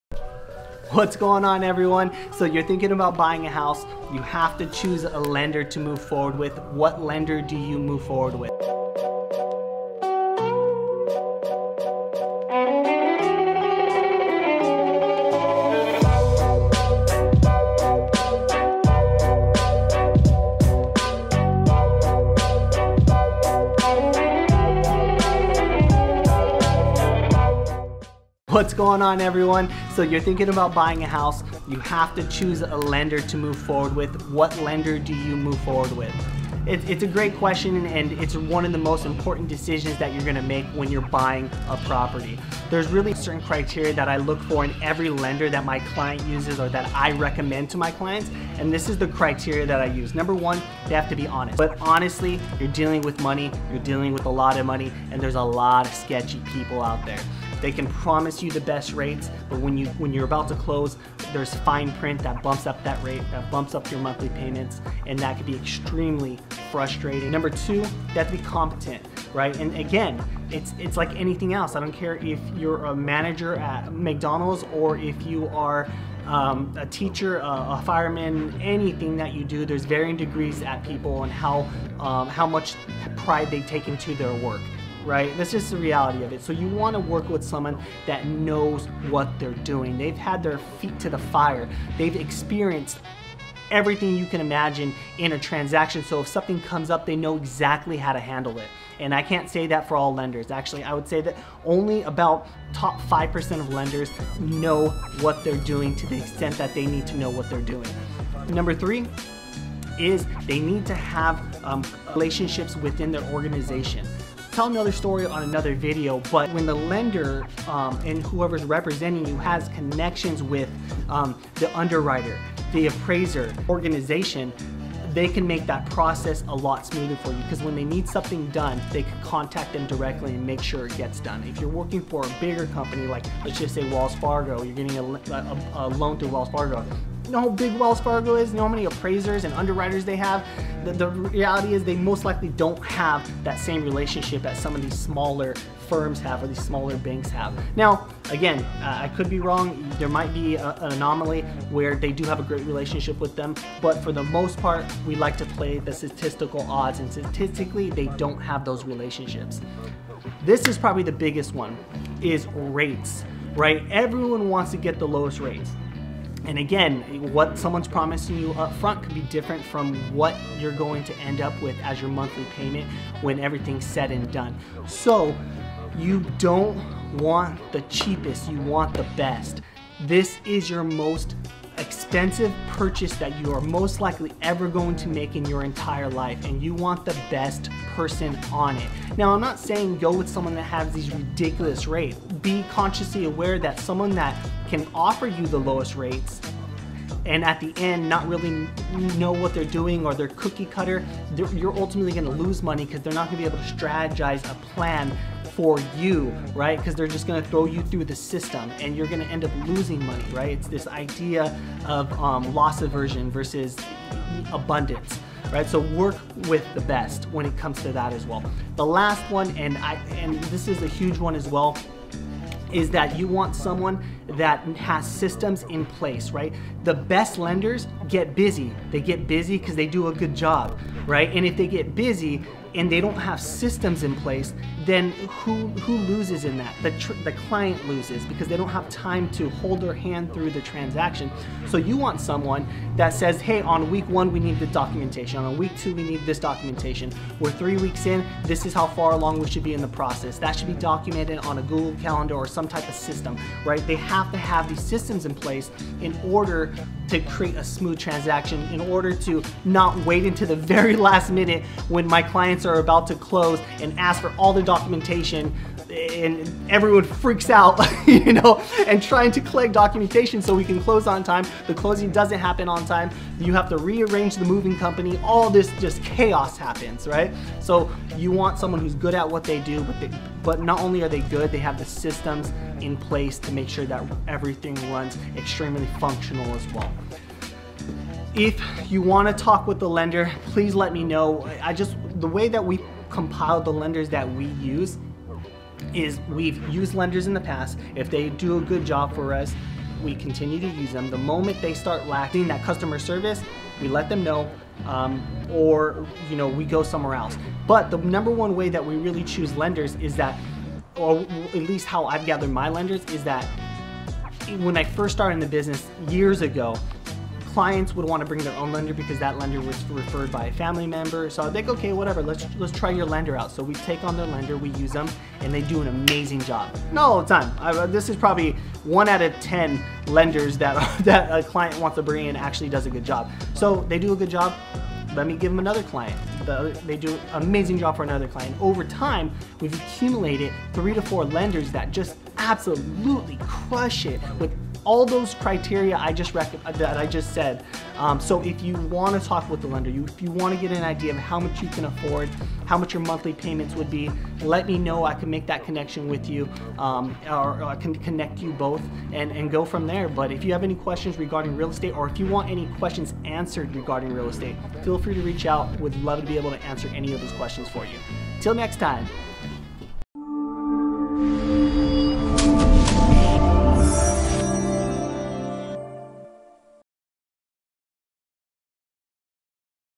5 Tips on Choosing a Lender - ALWAYS REMEMBER!
Why it’s important to choose your own lender when buying a home?

Experienced real estate agents typically have a line-up of professionals they like to work with, including lenders. It’s easy to take a recommendation from an agent you’ve grown close to, particularly since buying a home and getting ready to move are so stressful! With a tight time frame to get all requirements and contracts completed before the settlement date, having people on your side with an established connection can make things run faster and smoother.

Intro: (0:00)
Three Criteria's: (0:50)
Rates: (2:40)
Systems In Place: (3:40)